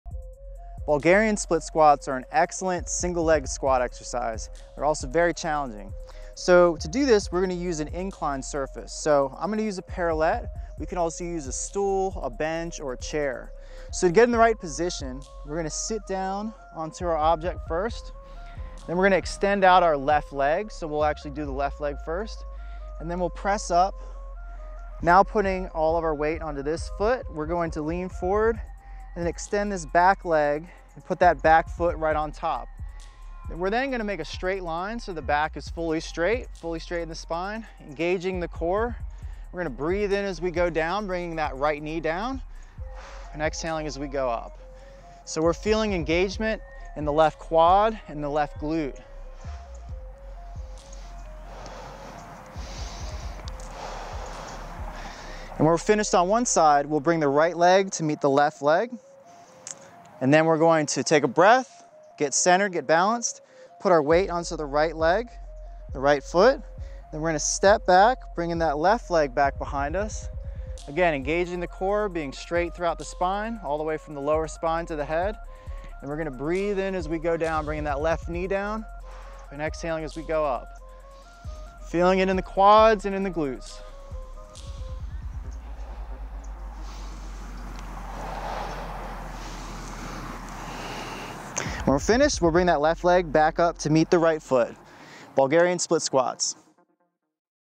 Bulgarian Split Squats
Bulgarian split squats are an excellent single leg squat exercise, and they're also very challenging. So to do this we're going to use an incline surface, such as a stool, a bench, or a chair.

To get in the right position, we're going to sit down onto our object first. Then we're going to extend out our left leg. So we'll actually do the left leg first and then we'll press up. Now putting all of our weight onto this foot we're going to lean forward and extend this back leg and put that back foot right on top.

We're then going to make a straight line so the back is fully straight in the spine while engaging the core. We're going to breathe in as we go down, bringing that right knee down and exhaling as we go up.

We're looking to feel the engagement in the left quad and the left glute. And once we're finished on one side,  we'll bring the right leg to meet the left leg. We'r then going to take a breath, get centered, get balanced, and put our weight onto the right leg and foot. We're then going to step back, bringing that left leg back behind us.

Again engaging the core, being straight throughout the spine, all the way from the lower spine to the head. We're then going to breathe in as we go down, bringing that left knee down and exhaling as we go up, feeling it in the quads and in the glutes.

When we're finished, we'll bring that left leg back up to meet the right foot. Repeat for reps!